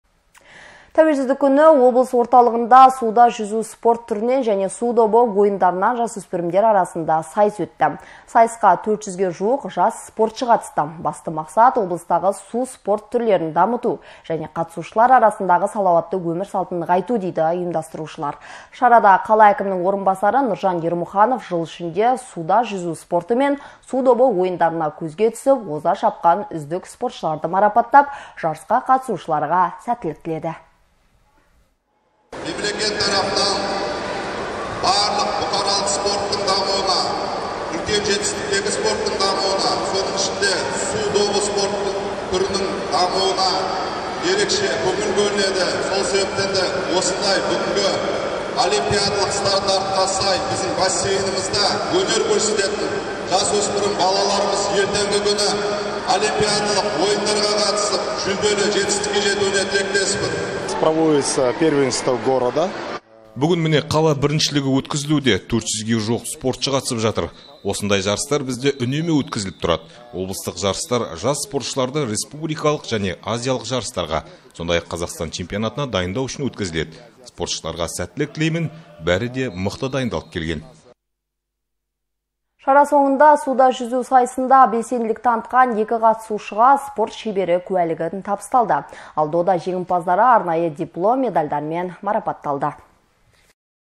СУ СПОРТЫНАН САЙЫС ӨТТІ
Тәуелсіздік күні облыс орталығында  суда жүзу спорт түрінен және су добы ойындарынан жасөспірімдер арасында сайыс өтті. Жарысқа 400-ге жуық жас спортшы қатысты. Басты мақсат - облыстағы су спорт түрлерін дамыту және қатысушылар арасындағы салауатты өмір салтын нығайту дейді ұйымдастырушылар. Шарада қала әкімінің орынбасары Нұржан Ермұханов жыл ішінде суда жүзу спорты мен су добы ойындарында көзге түсіп, оза шапқан үздік спортшыларды марапаттап, жарысқа қатысушыларға сәттілік тіледі.